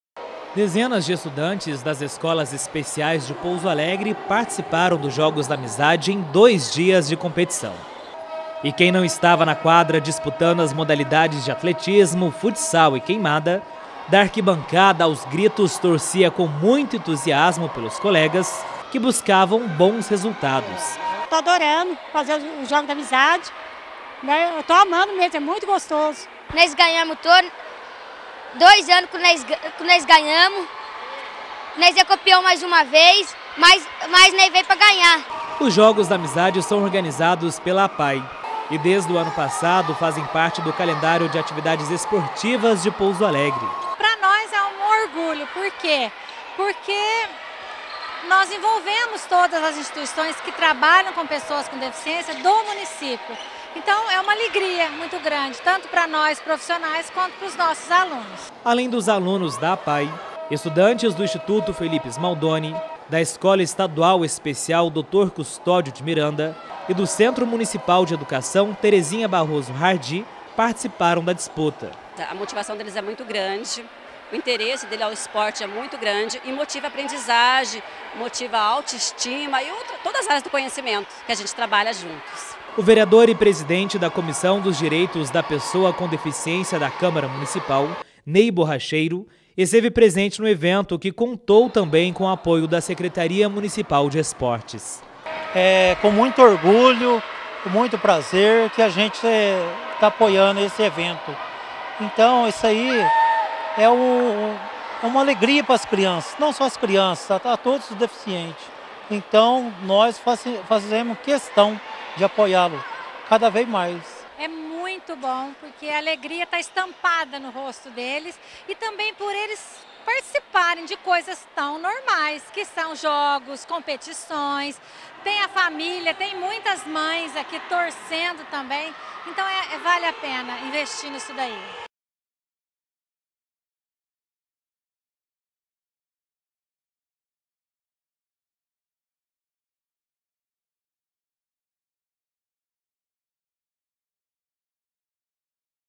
Jogos da Amizade reuniu estudantes de escolas especiais de Pouso Alegre
Dezenas de estudantes das escolas de ensino especial de Pouso Alegre participaram da oitava edição dos Jogos da Amizade, na Praça de Esportes. A competição faz parte das atividades da Semana da Pessoa com Deficiência Intelectual e Múltipla.